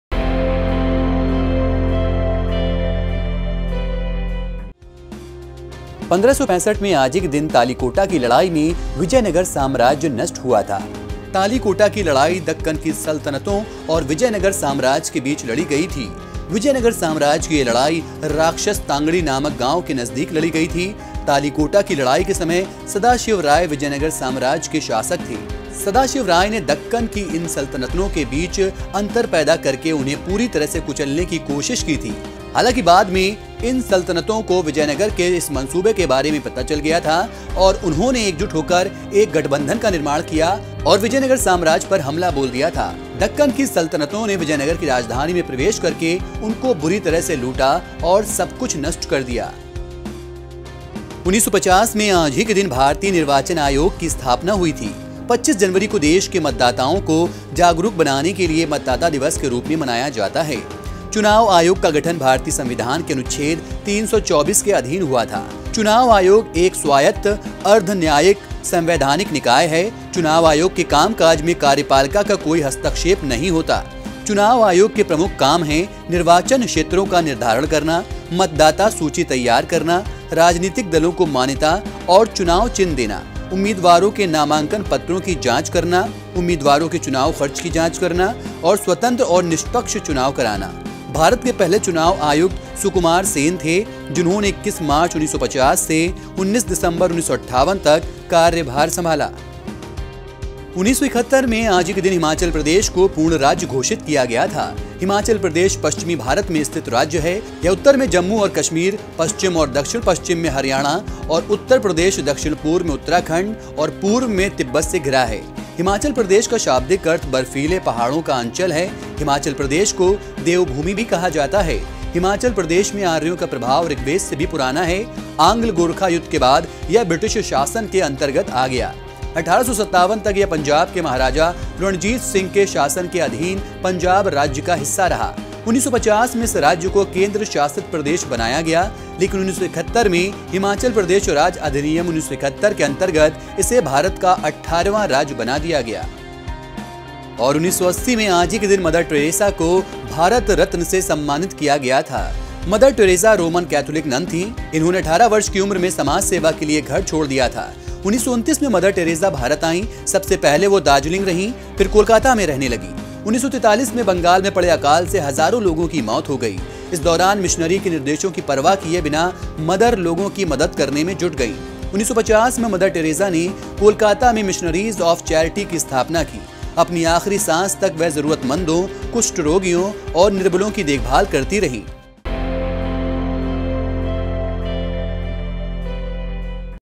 DB LIVE | 25 JAN 2017 | Today's History
1565 में तेल्लीकोटा की लड़ाई में विजयनगर साम्राज्य नष्ट हुआ। तालीकोटा की लड़ाई दक्कन की सल्तनतों और विजयनगर साम्राज्य के बीच लड़ी गई थी। विजयनगर साम्राज्य की यह लड़ाई राक्षस-तांगड़ी नामक गांव के नजदीक लड़ी गई थी। तालीकोटा की लड़ाई के समय, सदाशिव राय विजयनगर साम्राज्य के शासक थे। सदाशिव राय नें दक्कन की इन सल्तनतों के बीच अंतर पैदा करके उन्हें पूरी तरह से कुचलने की कोशिश की थी। हालांकि बाद में इन सल्तनतों को विजयनगर के इस मंसूबे के बारे में पता चल गया था और उन्होंने एकजुट होकर एक गठबंधन का निर्माण किया था और विजयनगर साम्राज्य पर हमला बोल दिया था। दक्कन की सल्तनतों ने विजयनगर की राजधानी में प्रवेश करके उनको बुरी तरह से लूटा और सब कुछ नष्ट कर दिया।

1950 में भारतीय निर्वाचन आयोग की स्थापना हुई थी। 25 जनवरी को देश के मतदाताओं को जागरुक बनाने के लिए मतदाता दिवस के रूप में मनाया जाता है। चुनाव आयोग का गठन भारतीय संविधान के अनुच्छेद 324 के अधीन हुआ था। चुनाव आयोग एक स्वायत्त, अर्द्ध न्यायिक, संवैधानिक निकाय है। चुनाव आयोग के काम काज में कार्यपालिका का कोई हस्तक्षेप नहीं होता। चुनाव आयोग के प्रमुख काम हैं निर्वाचन क्षेत्रों का निर्धारण करना, मतदाता सूची तैयार करना, राजनीतिक दलों को मान्यता और चुनाव चिह्न देना, उम्मीदवारों के नामांकन पत्रों की जांच करना, उम्मीदवारों के चुनाव खर्च की जांच करना और स्वतंत्र और निष्पक्ष चुनाव कराना। भारत के पहले चुनाव आयुक्त सुकुमार सेन थे, जिन्होंने 21 मार्च 1950 से 19 दिसंबर 1958 तक कार्यभार संभाला।

1971 में हिमाचल प्रदेश को पूर्ण राज्य घोषित किया गया था। हिमाचल प्रदेश पश्चिमी भारत में स्थित राज्य है। यह उत्तर में जम्मू और कश्मीर, पश्चिम और दक्षिण-पश्चिम में हरियाणा और उत्तर प्रदेश, दक्षिण-पूर्व में उत्तराखंड और पूर्व में तिब्बत से घिरा है। 'हिमाचल' प्रदेश का शाब्दिक अर्थ बर्फ़ीले पहाड़ों का अंचल' है। हिमाचल प्रदेश को देव भूमि भी कहा जाता है। हिमाचल प्रदेश में आर्यों का प्रभाव ऋग्वेद से भी पुराना है। आंग्ल-गोरखा युद्ध के बाद, यह ब्रिटिश शासन के अंतर्गत आ गया। 1857 तक यह पंजाब के महाराजा रणजीत सिंह के शासन के अधीन पंजाब राज्य का हिस्सा रहा। 1950 में इस राज्य को केन्द्र शासित प्रदेश बनाया गया, लेकिन 1971 में 'हिमाचल प्रदेश राज्य अधिनियम-1971' के अन्तर्गत इसे भारत का अठारहवां राज्य बना दिया गया।

1980 ‌में मदर टेरेसा को भारत रत्न से सम्मानित किया गया था। मदर टेरेसा रोमन कैथोलिक नन थीं। इन्होंने 18 वर्ष की उम्र में समाजसेवा के लिए घर छोड़ दिया था। 1929 में मदर टेरेसा भारत आईं। सबसे पहले वो दार्जिलिंग रहीं, फिर कोलकाता में रहने लगीं। 1943 में बंगाल में पड़े अकाल से हज़ारों लोगों की मौत हो गई। इस दौरान मिशनरी के निर्देशों की परवाह किए बिना मदर लोगों की मदद करने में जुट गईं। 1950 में मदर टेरेसा ने कोलकाता में मिशनरीज ऑफ चैरिटी की स्थापना की। अपनी आखिरी सांस तक वह जरूरतमंदों, कुष्ठ रोगियों और निर्बलों की देखभाल करती रहीं। 1979 में मदर टेरेसा को नोबेल शांति पुरस्कार से सम्मानित किया गया।